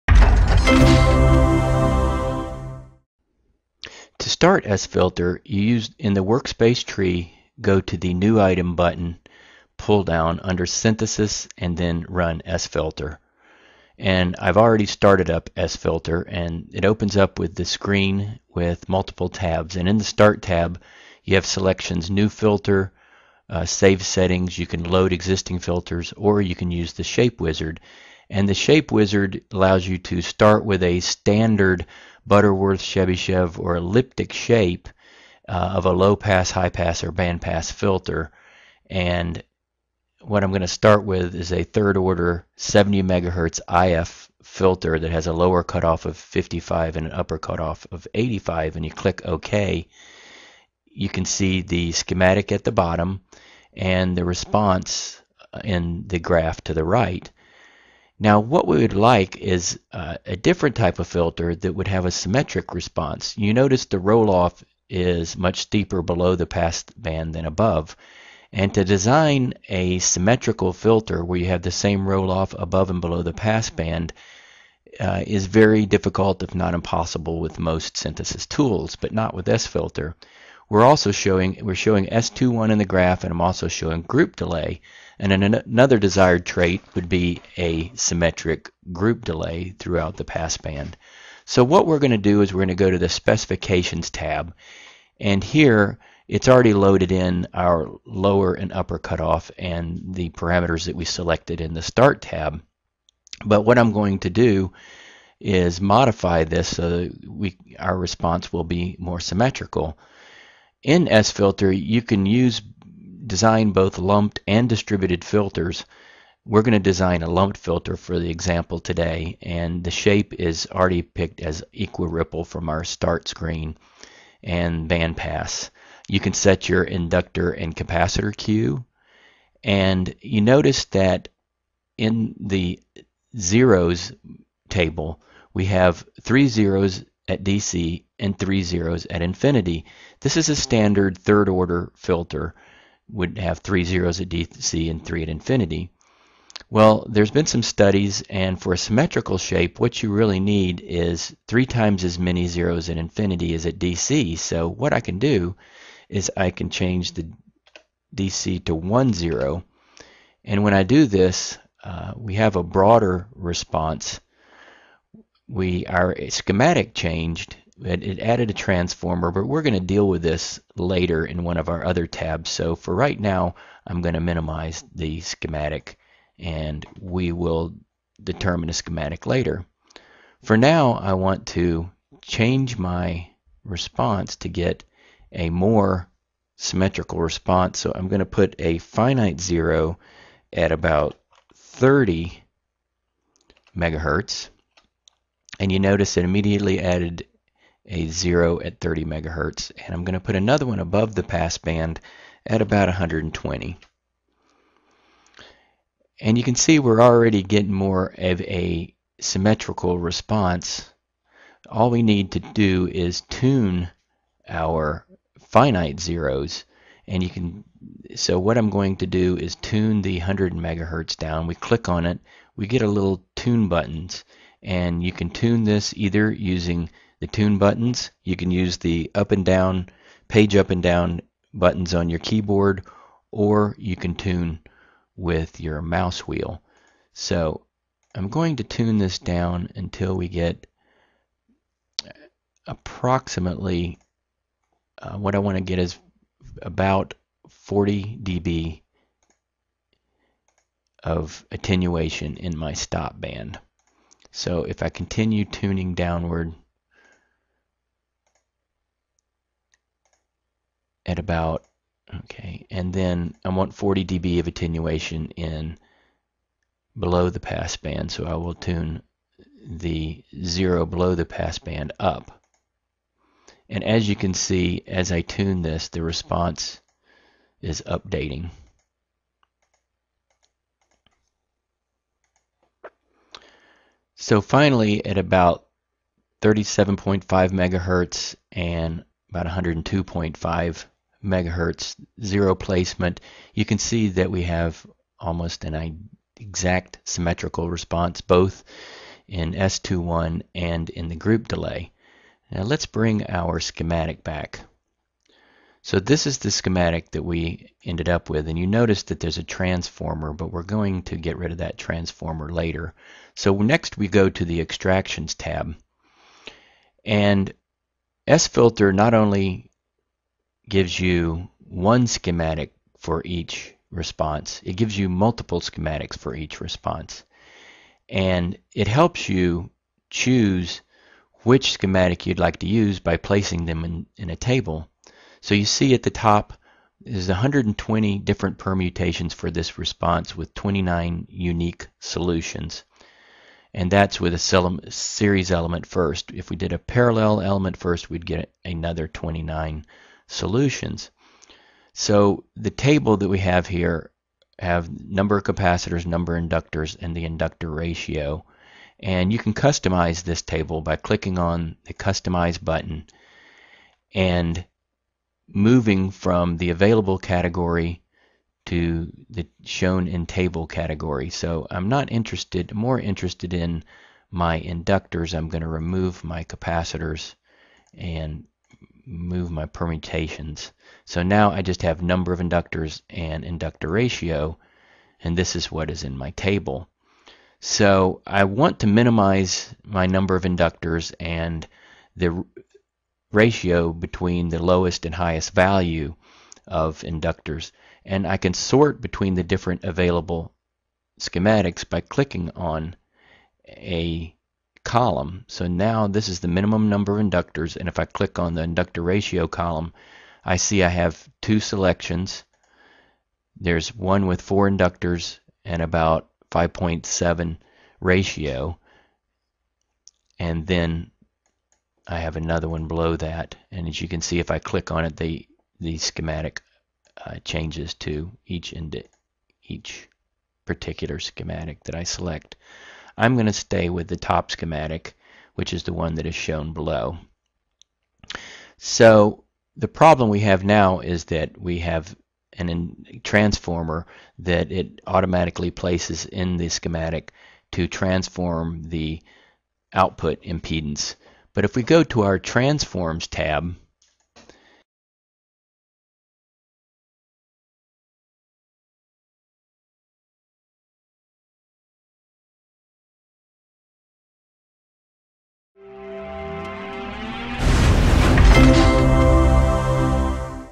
Genesys S/Filter Advanced Direct Filter Synthesis - Part 1a
S/Filter is actually three filter synthesis tools: Filter for lumped LC Filters, M/Filter for distributed filters, and S/Filter for both LC and distributed. The difference is Filter and M/Filter are limited to standard shapes such as Butterworth and Cheby; whereas, S/Filter allows the designer freedom to design custom filter shapes. Designers can customize a response by placing transmission zeros where desired. This video covers just the S/Filter module. View "Genesys S/Filter Advanced Direct Filter Synthesis - Part 1b" to complete this video.